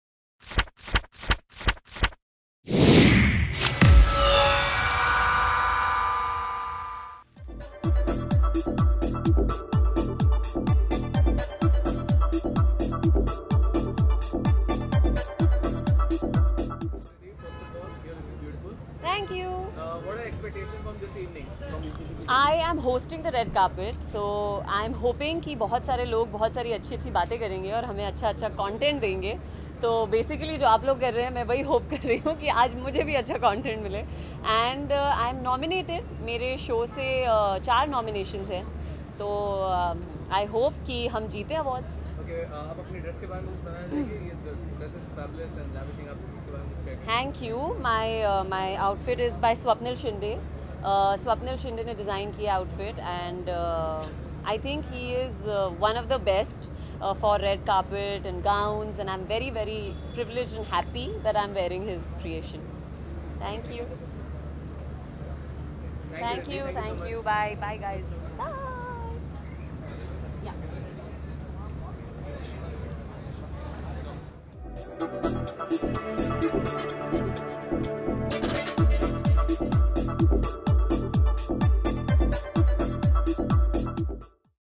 Riddhi Dogra's sexy outfit steals the show @ Zee Gold Awards 2017
Watch Tv actress Riddhi Dogra looking gorgeous at Zee Gold Awards 2017